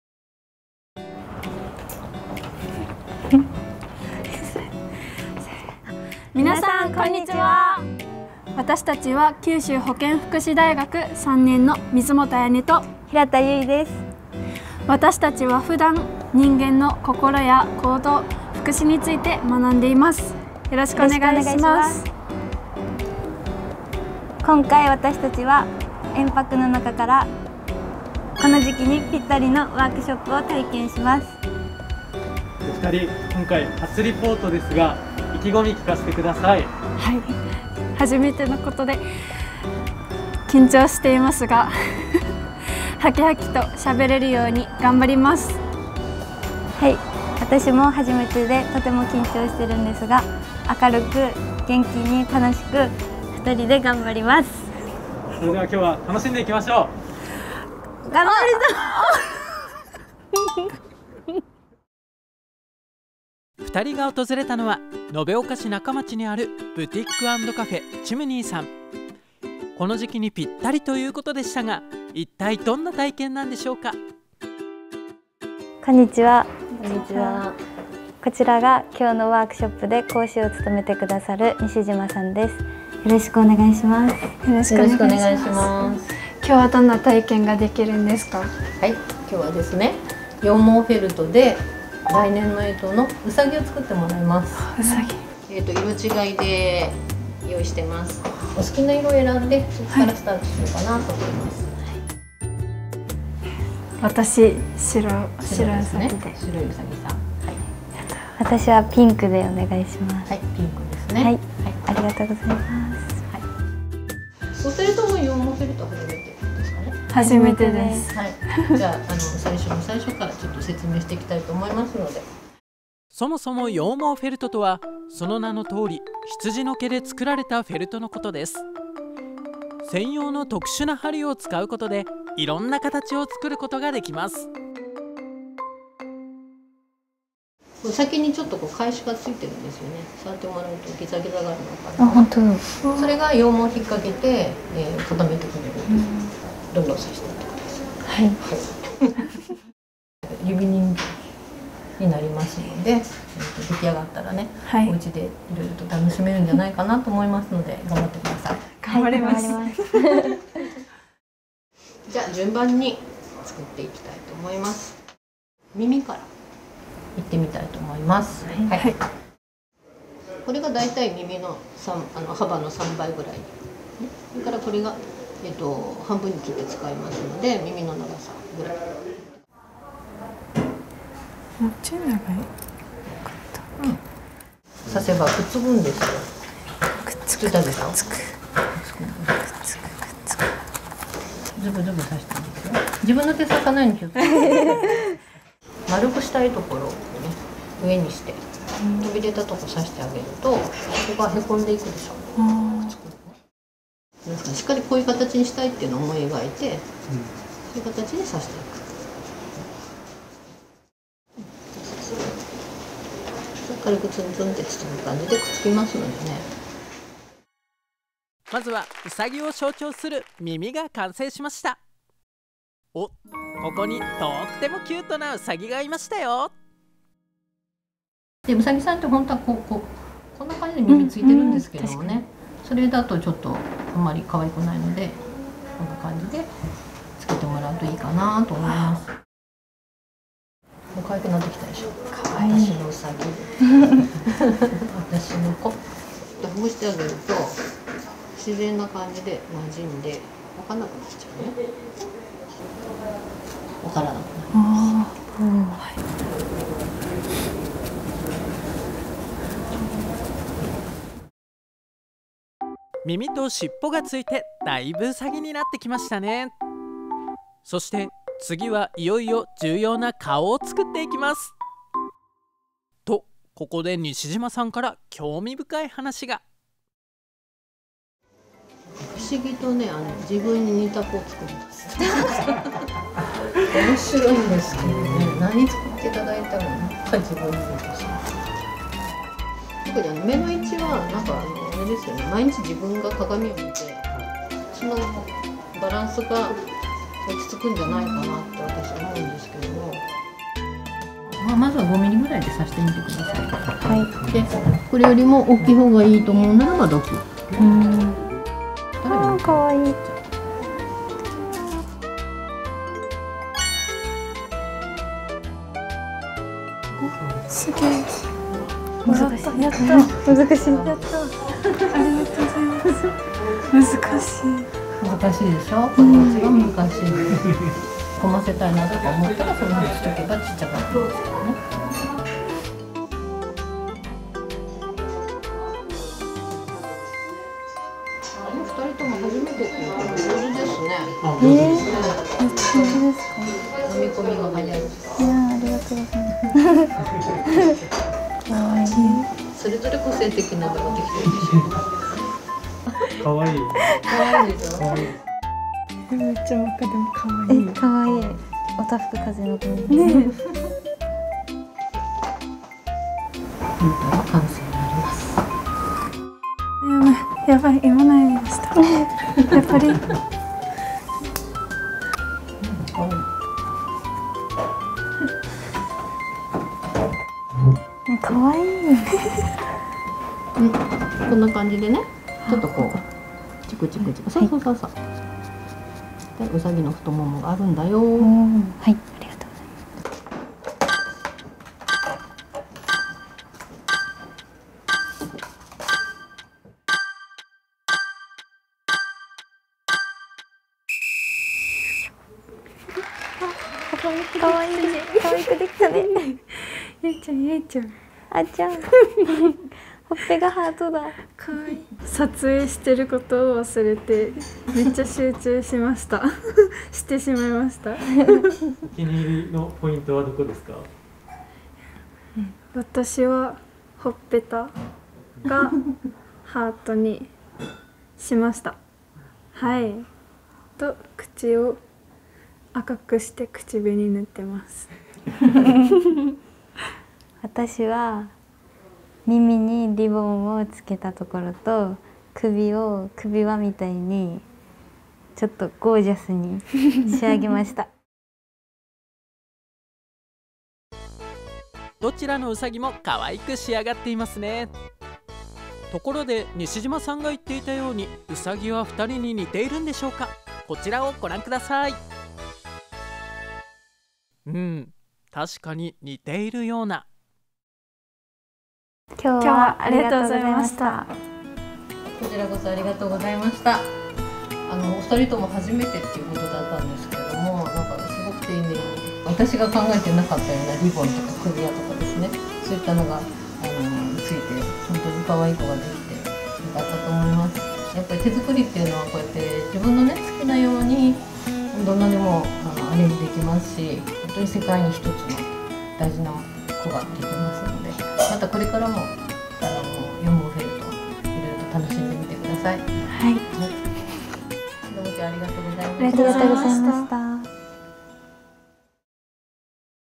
【特集】大学生が羊毛フェルトに挑戦 (2022年12月15日 放送)
地元にある様々な地域資源を「プログラム」という形で体験してもらうことで、地域の価値・魅力を高め、まちを元気にする取り組みである「えんぱく」
そのえんぱくの中で、今の時期にピッタリのプログラムを大学生が体験してきました。
今回が初リポートの大学生。どんな内容となったのでしょうかお楽しみに！